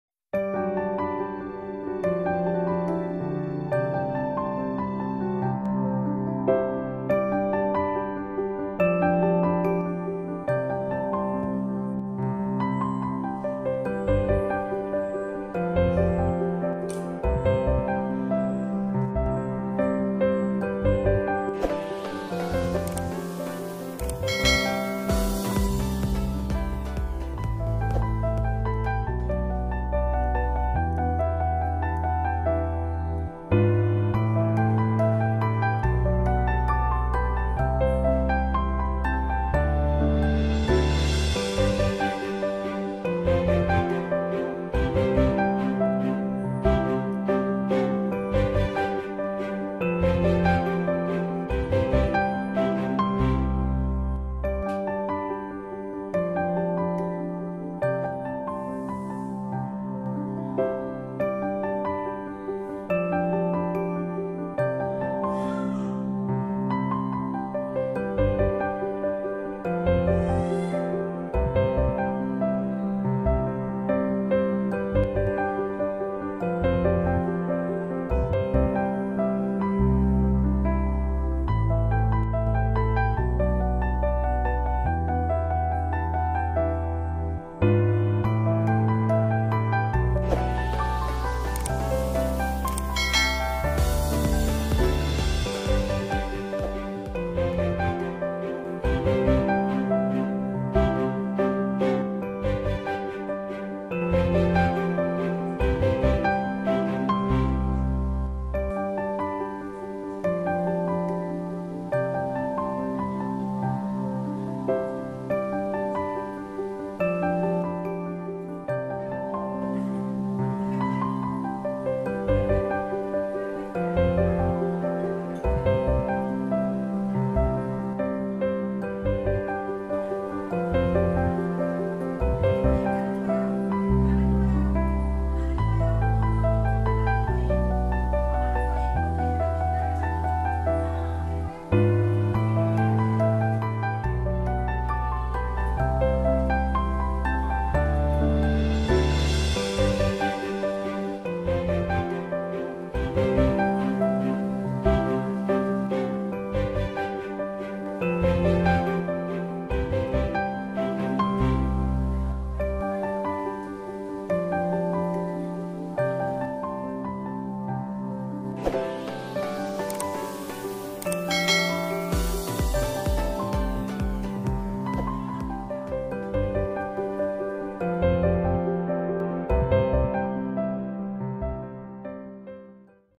Water Stone Craft DIY
Draw an Owl in a Water stone using fabric paints